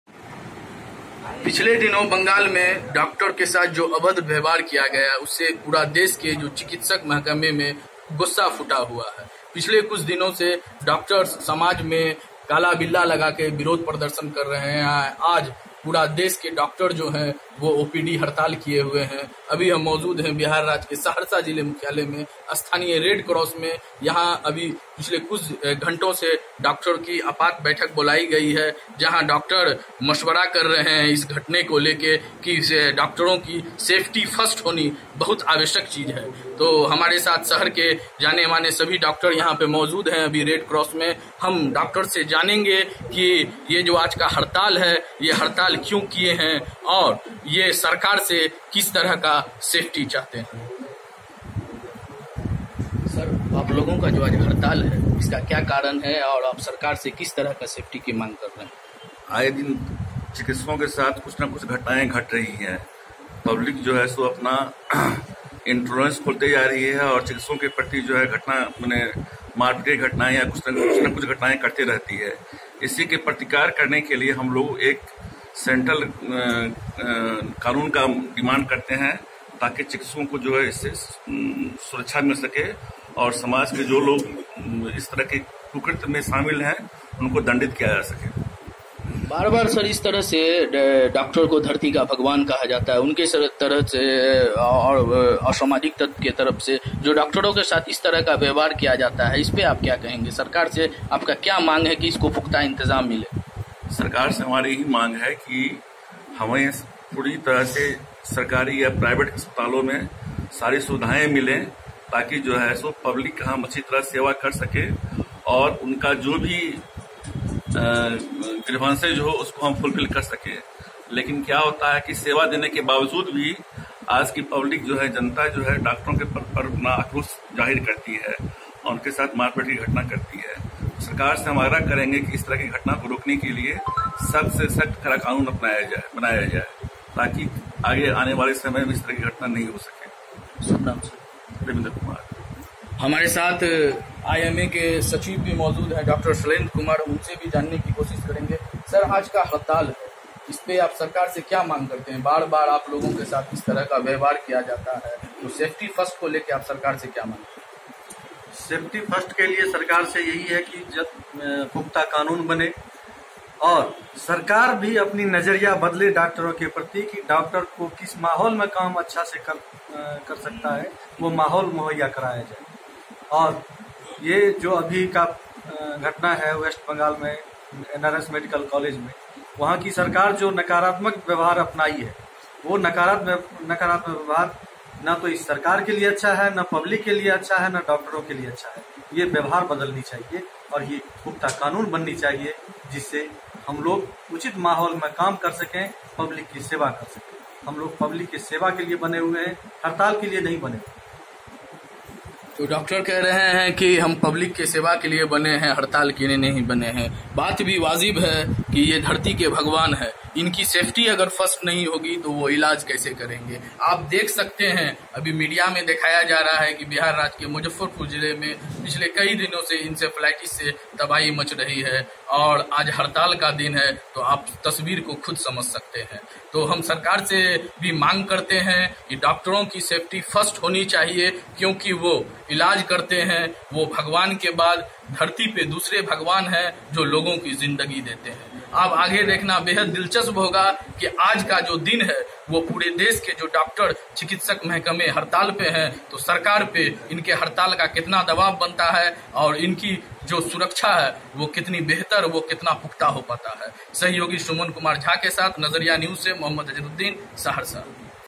जरूर सुनें हड़ताल पर है,चिकित्सक।उन्होंने सरकार से क्या कहा.....
बंगाल में डॉक्टर के साथ अभद्र व्यवहार से पूरा डॉक्टर समाज गुस्से में है।पिछलें कई दिनों से पूरे देश के डॉक्टर काला बिल्ला लगाकर विरोध प्रदर्शन कियें।वहीं आज सरकारी और निजी अस्पताल के डॉक्टर हड़ताल कर विरोध प्रदर्शन कर सरकार से सेफ्टी फर्स्ट की मांग कर रहें है।आज सहरसा जिला मुख्यालय के रेड क्रोस सभागार में डॉक्टरों की बैठक आयोजित किया गया।जिसमें शहर के कई दर्जन डॉक्टर मौजूद थे।